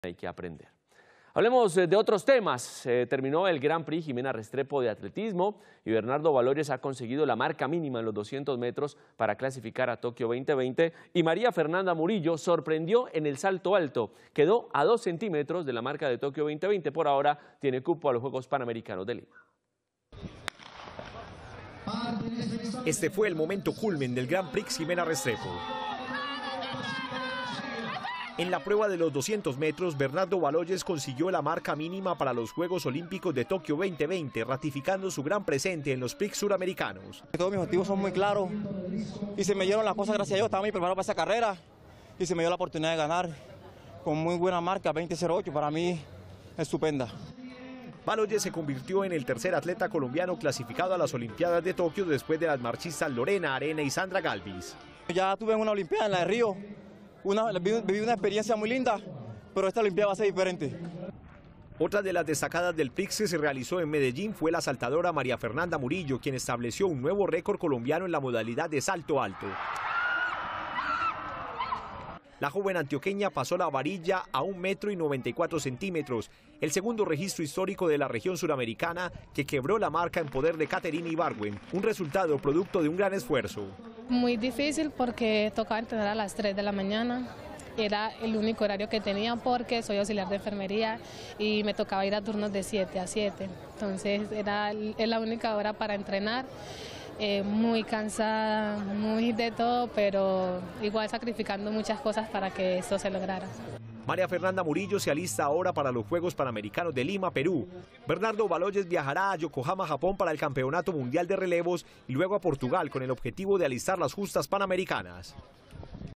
Atletas colombianos se destacan en los Prix Sudamericanos [Noticias] - Telemedellín
En la prueba de los 200 metros, Bernardo Baloyes consiguió la marca mínima para los Juegos Olímpicos de Tokio 2020 ratificando su gran presente en los Prix Sudamericanos.